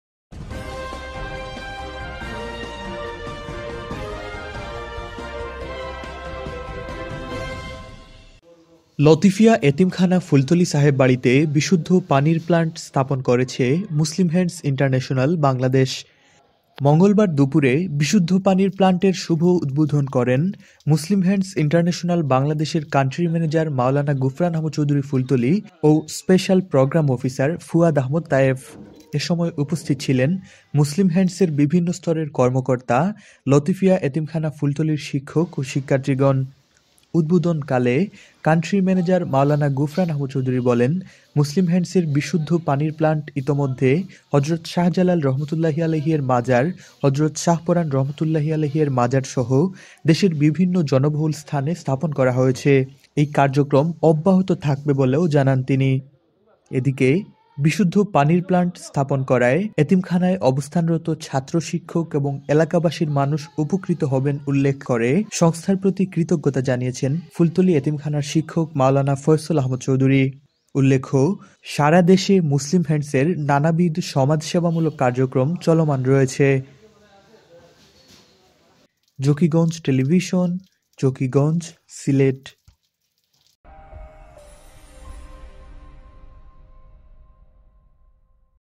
লতিফিয়া এতিমখানা ফুলতলীতে বিশুদ্ধ পানির প্লান্ট স্থাপন করলো মুসলিম হ্যান্ডস ইন্টারন্যাশনাল বাংলাদেশ
লতিফিয়া এতিমখানা ফুলতলী ছাহেব বাড়ীতে বিশুদ্ধ পানির প্লান্ট স্থাপন করলো মুসলিম হ্যান্ডস ইন্টারন্যাশনাল বাংলাদেশ।